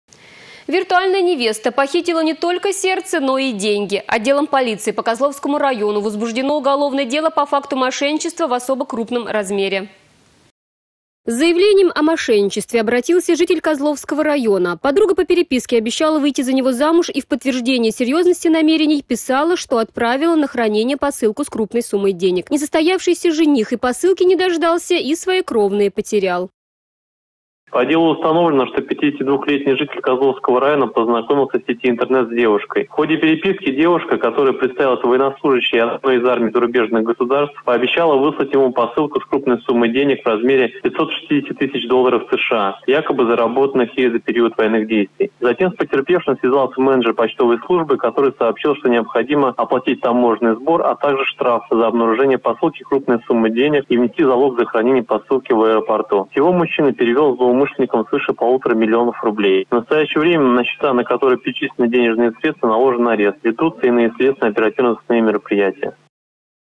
Житель Козловского района перевел виртуальной невесте более 1,5 млн рублей
Виртуальная невеста похитила не только сердце, но и деньги.  Отделом полиции по Козловскому району возбуждено уголовное дело по факту мошенничества в особо крупном размере.